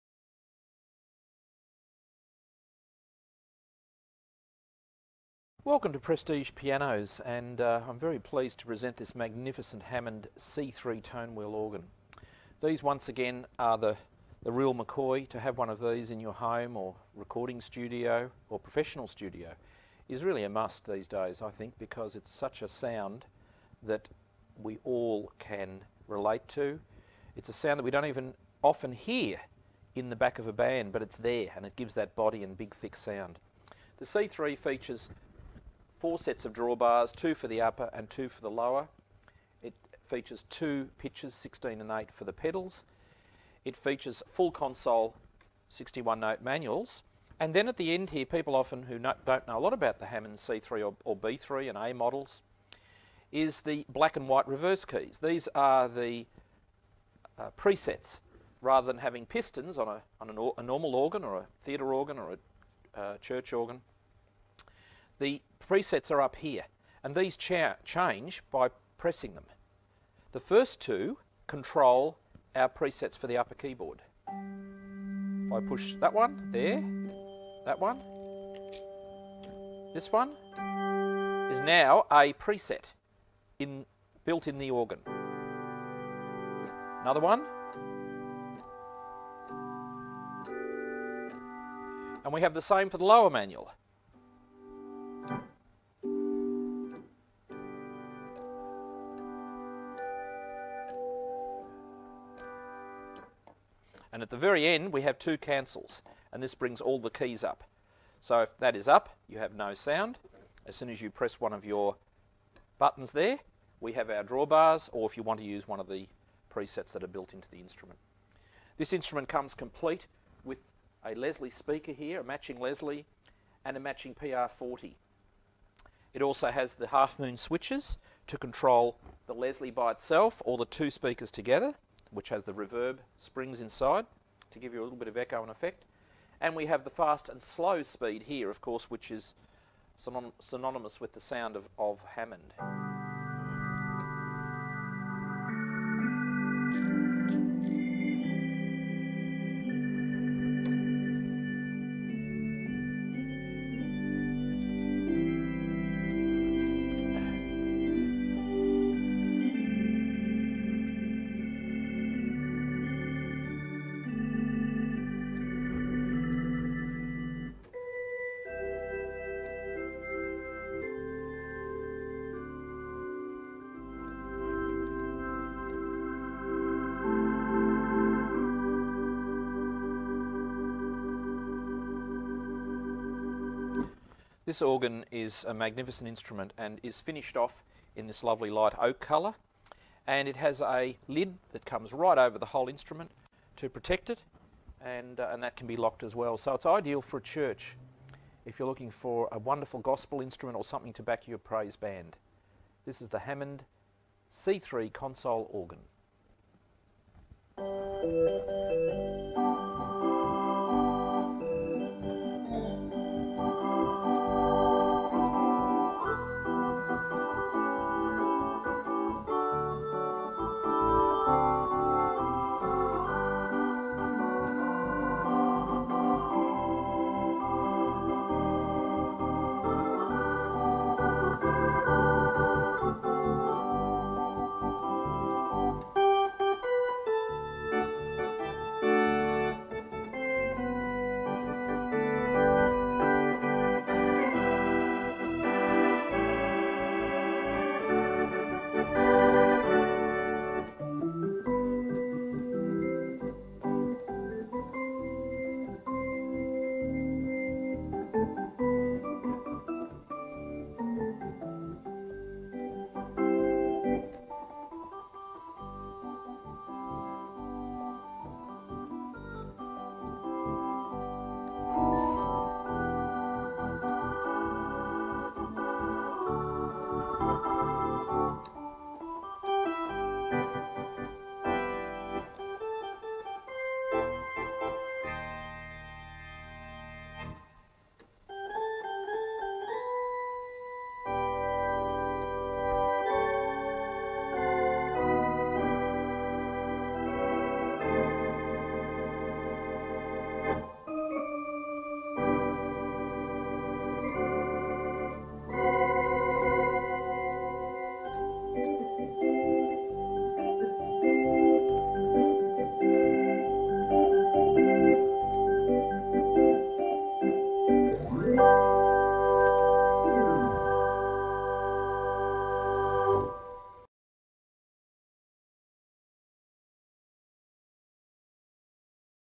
Hammond C3 Tone Wheel console organ
Just arrived - Hammond C-3 organ.

Hammond C-3 Console Organ, with PR40 speaker cabinet and Leslie 122 RV  speaker tone cabinet.

If you love the sound and feel of a real tone wheel organ, then look no further! Here is a beautiful C-3 (B-3 in a gothic cabinet) in light Oak. This organ would be fairly rare in this colour, plus it has the matching PR 40 cabinet and Leslie model 122 RV cabinet for that complete rotating full live sound. This organ is for the true enthusiast, who wants the sound and feel this beautiful beast can give! This particular one has been fully work shopped and overhauled as required. Ideal for the professional musician, home studio enthusiast, a church or just someone who wants the ultimate Hammond sound!